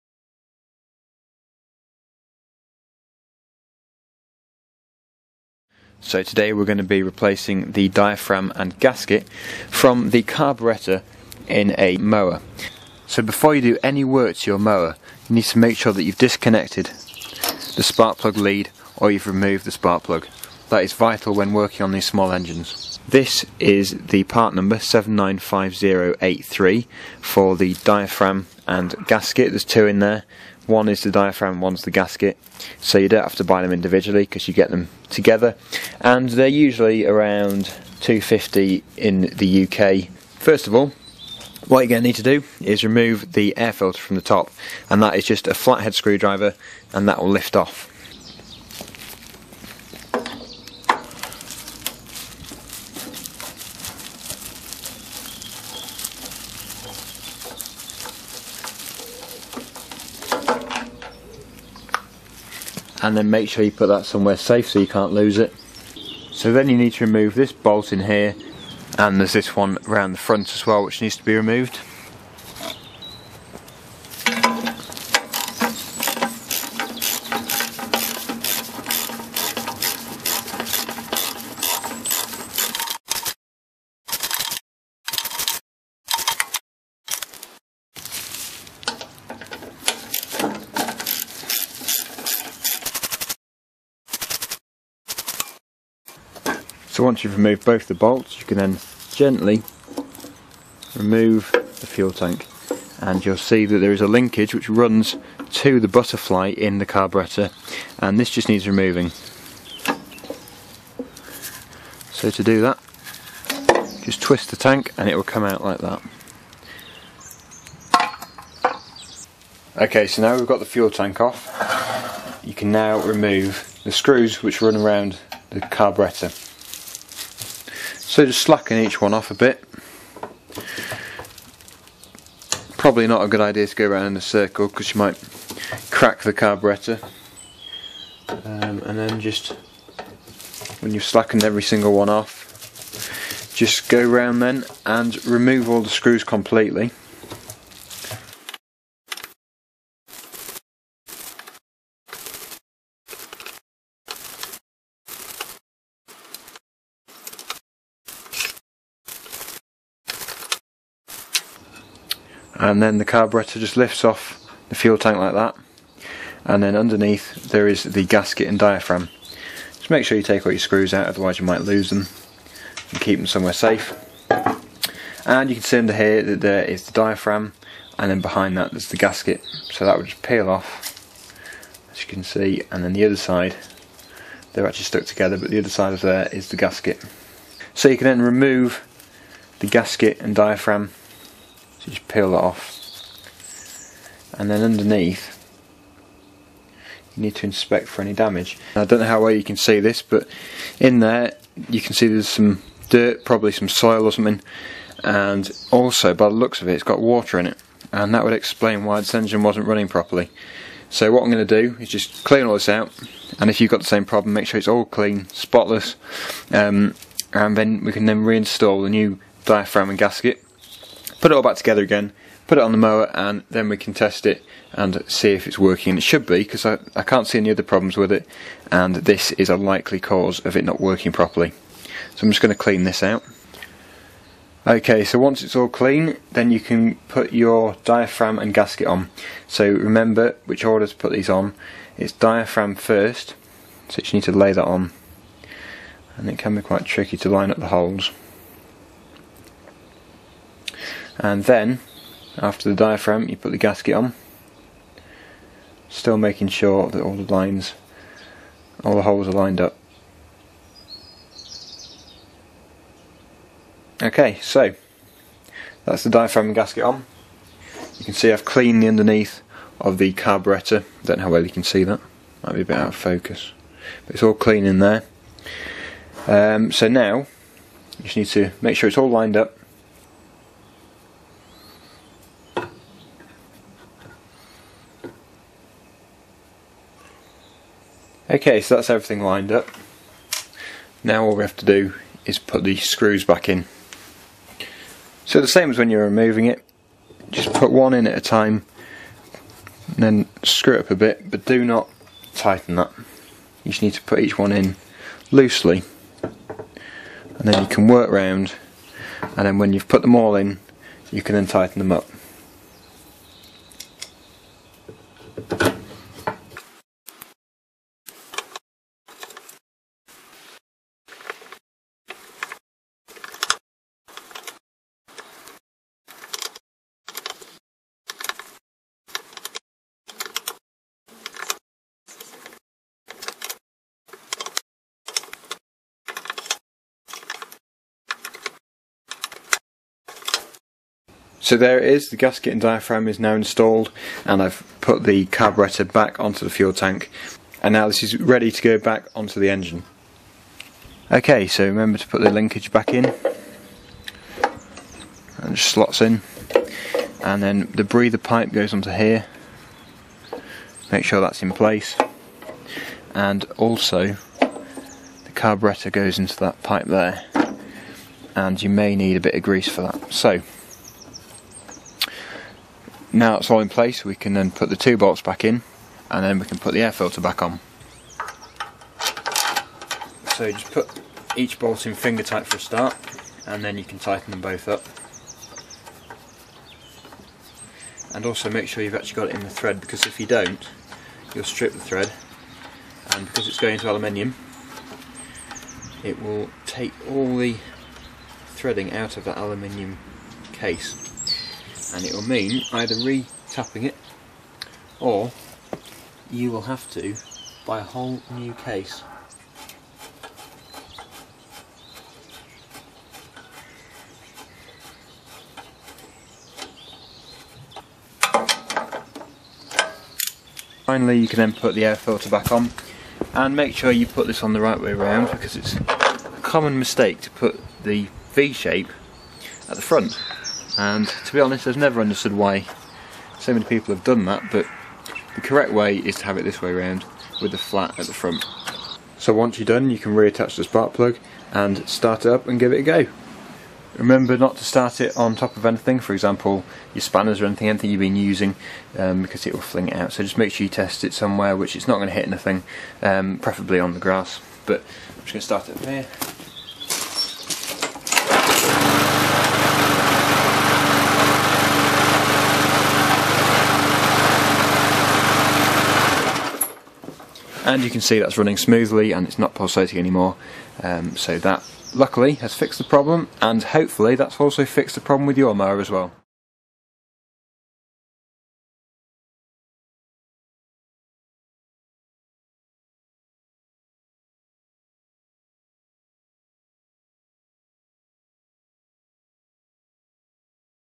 How to Replace Diaphragm and Gasket on Briggs and Stratton Engine Primer Carburetor
How to replace the diaphragm and gasket on a primer carburetor type Briggs and Stratton small lawn mower engine.